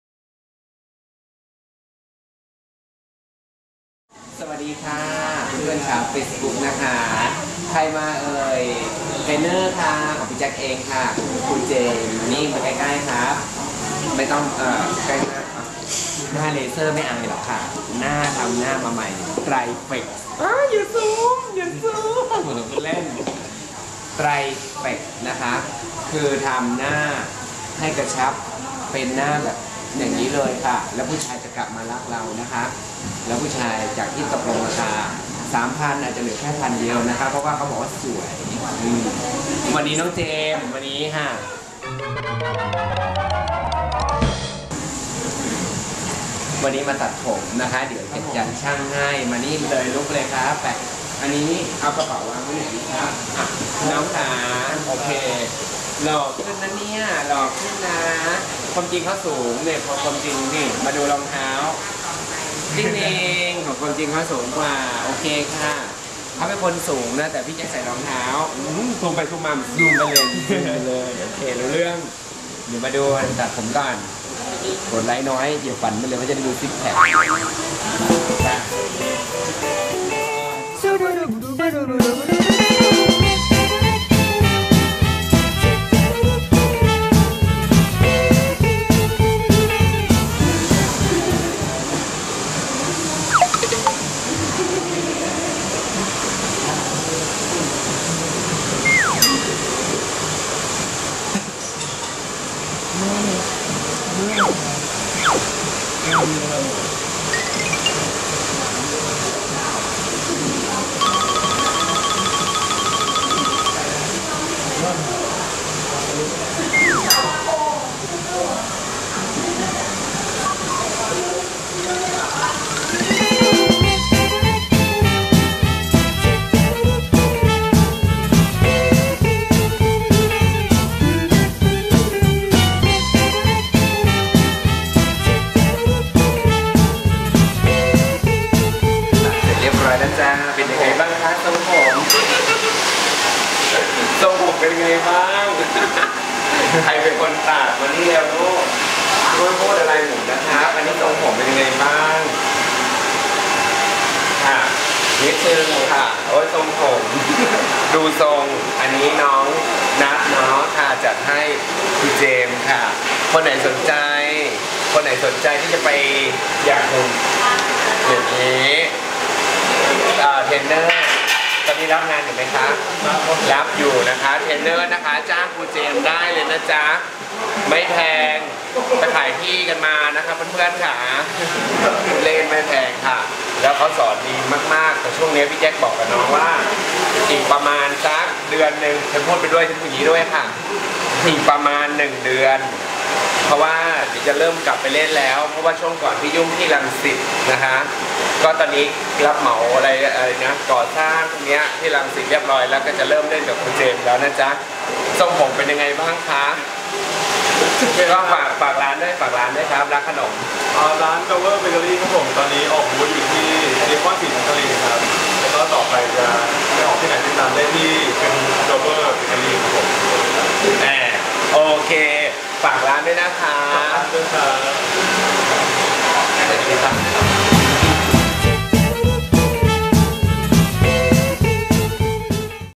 ครูเจมส์ มาตัดผมค่าา
ครูเจมส์ มาตัดผม ว่าที่ช่าง น้องนัท จัดเต็มค่ะ